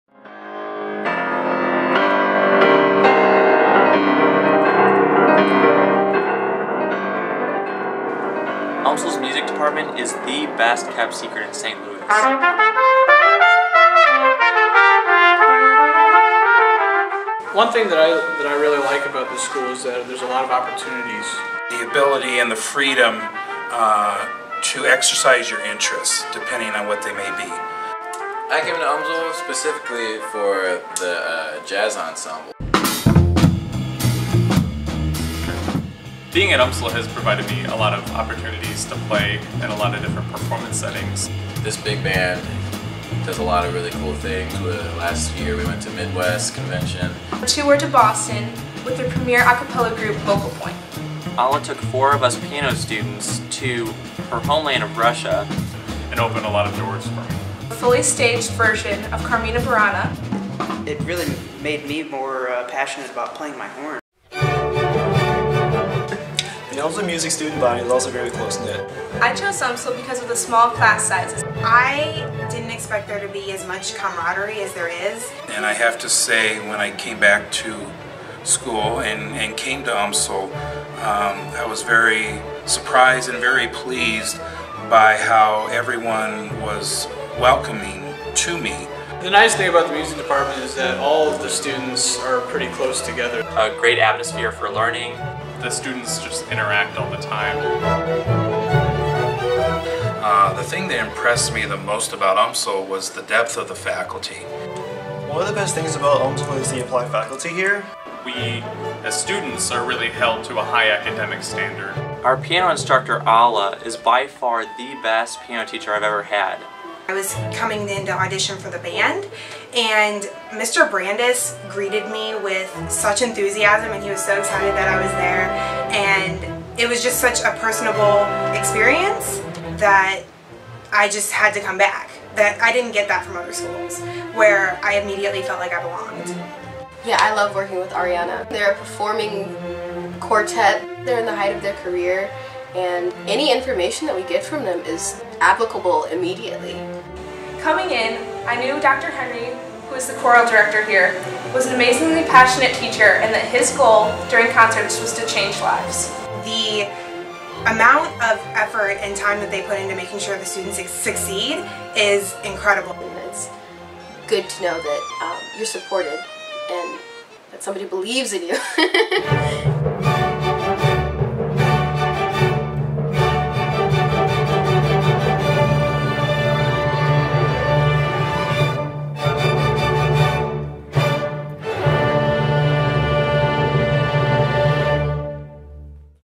UMSL Department of Music, Student Video
UMSL students tell why they decided to study music at UMSL.